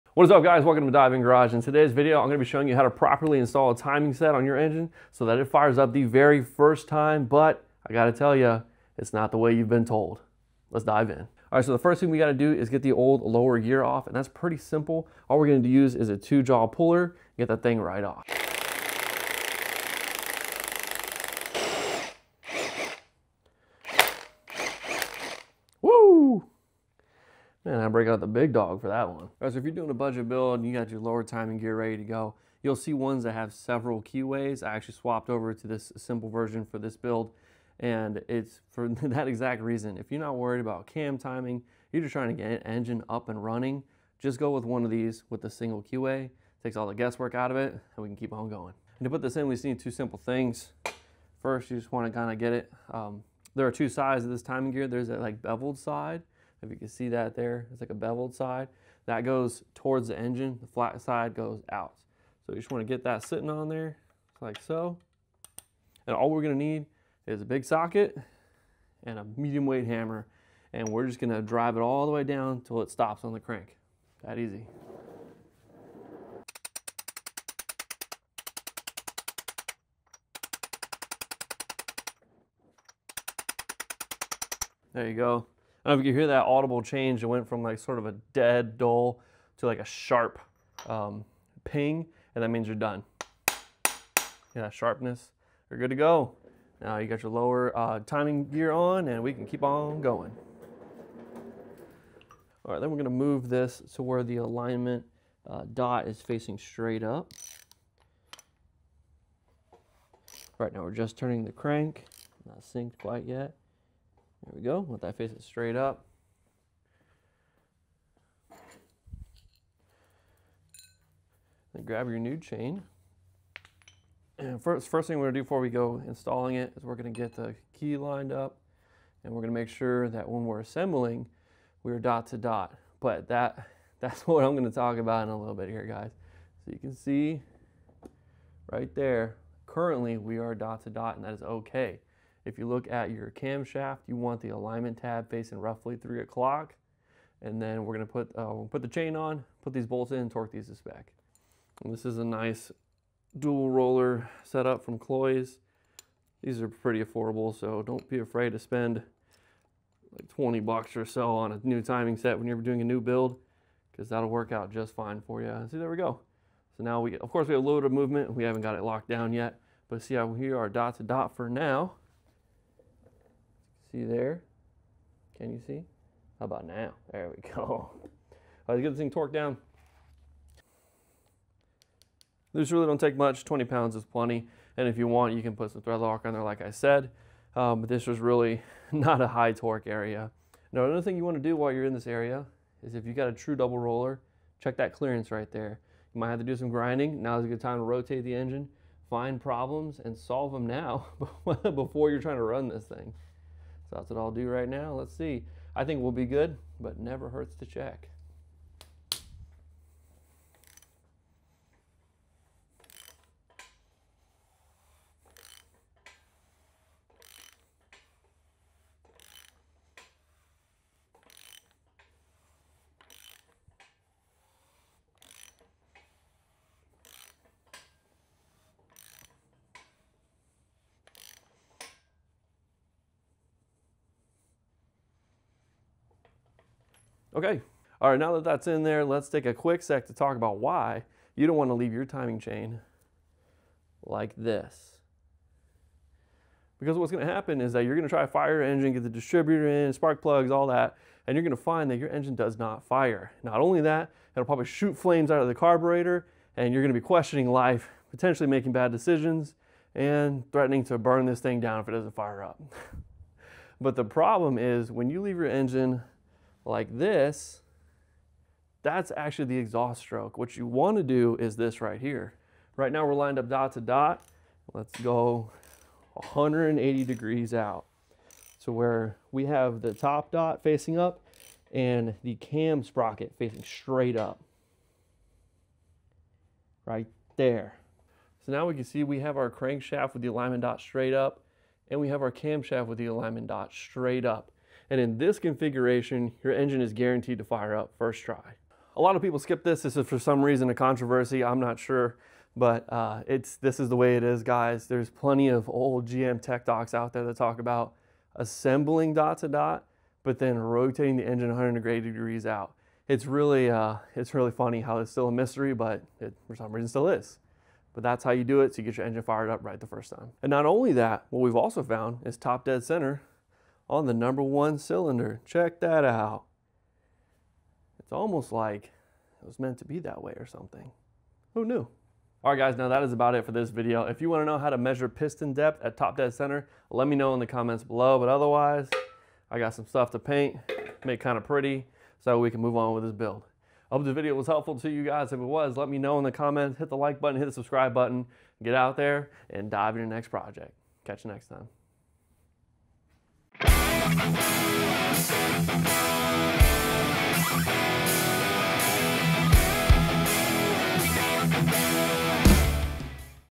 The Engine Building Timing Secret You Need to Know
In this video I show you how to install a timing chain on a Small Block Chevy (SBC), same process for a Big Block Chevy (BBC). Some tips and tricks along the way and the BIG reason why your fresh engine build is shooting flames instead of running like a monster!

My Camera Setup:
DJI Osmo Action 6: